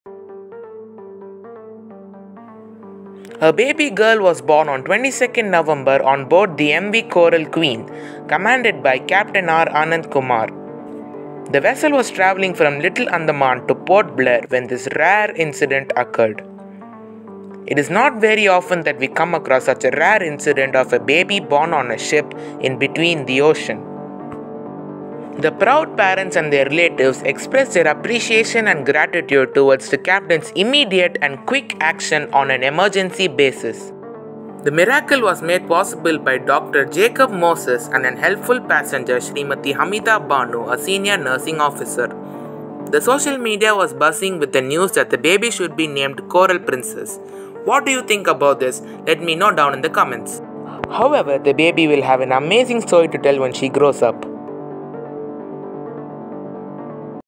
/Baby Girl Born on Board M.V.Coral Queen Ship.(for latest pics...check link in description)
These are the new and latest pics of Coral Queen Ship. In which the Baby was Born. (Aremiti Ferry is the old name, The new name is Coral Queen.)
Pls View this link for the Latest Pics:

/Baby Girl Born on Board M.V.Coral Queen Ship.

Its indeed the happiest moment for all the passengers who have traveled in the ship M V CORAL QUEEN on 22nd November 2020.

Yes, everyone could recall that moment, when an emergency arises for a woman who was pregnant and started feeling the labor pain and was clueless about it. As soon as the message conveyed to the Captain, he took immediate, quick, and emergency action, and the Ship's Doctor and a senior nurse, one of the passengers, helped the woman to deliver a beautiful baby girl on board.

It's one of the rarest incidents and a great record to remember life long, not only to the parents but also to all the passengers and crewmates of the ship.

The baby will be having a great time listening to her own story when she grows up.